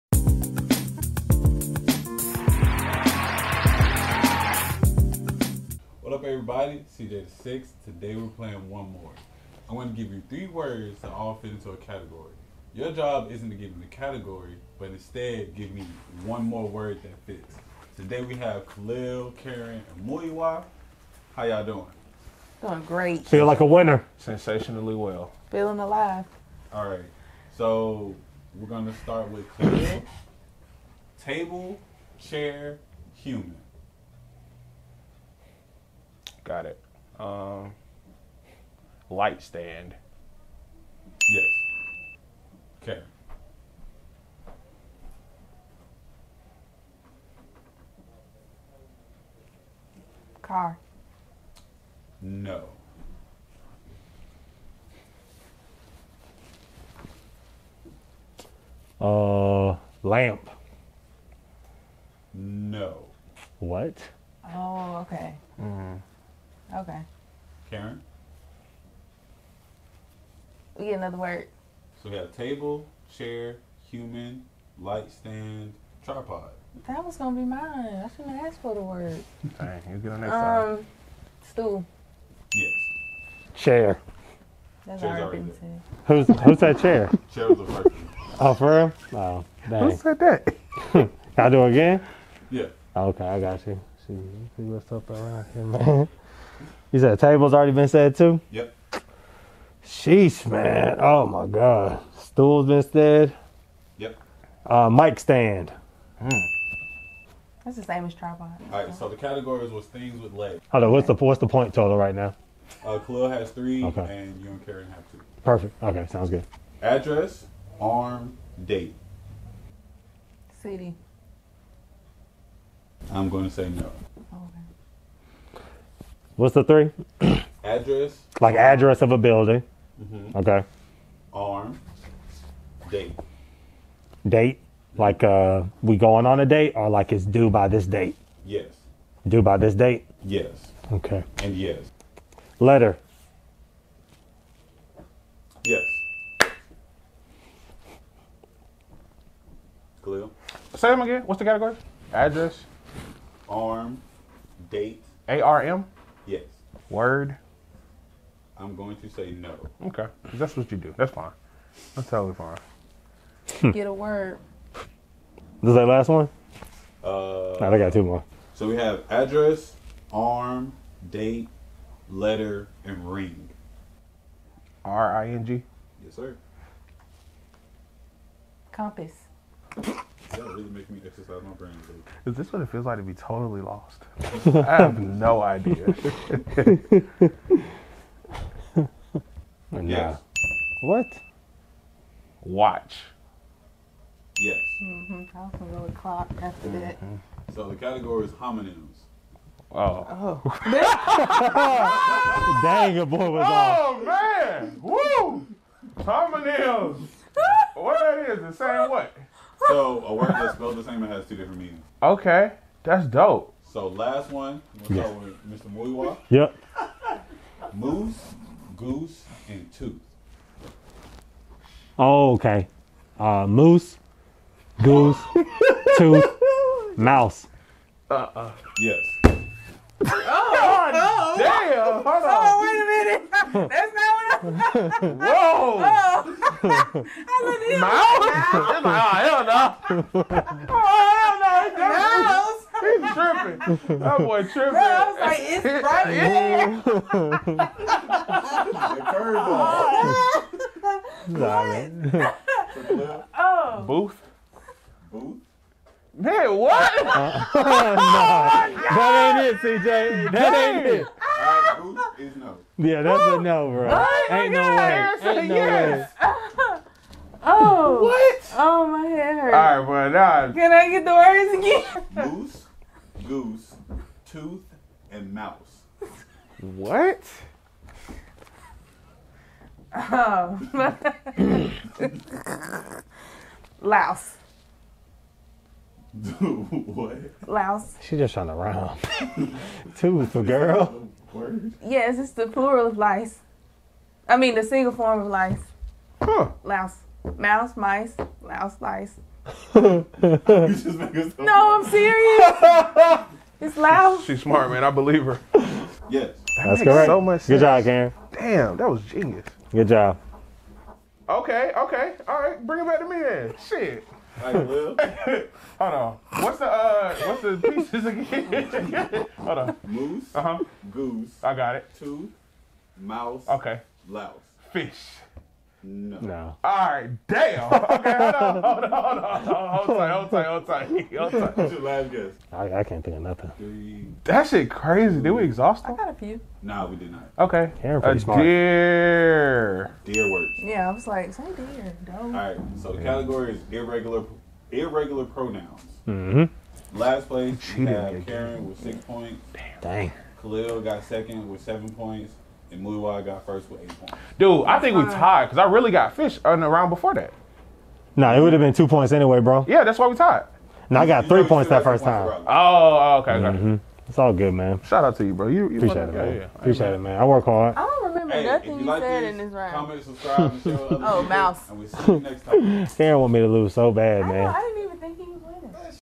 You Deserve Bragging Rights For Winning | One More Party Game | Game Nights @6IX Studios
Khalil, Muyiwa, and Caryn are back for a rematch. Now that they know the rules of the game, how will the outcome change? Do you think you can keep up with them? Let's find out on today's episode.

0:31 - Round 1
1:57 - Round 2
3:35 - Round 3
5:47 -  Results

Today we're playing a custom game called One More. You're given 3 words that all fit a theme. Your job isn't to tell what the theme is but instead, give another word that fits into the theme. However, you're only given a yes or no answer for each word that you guess.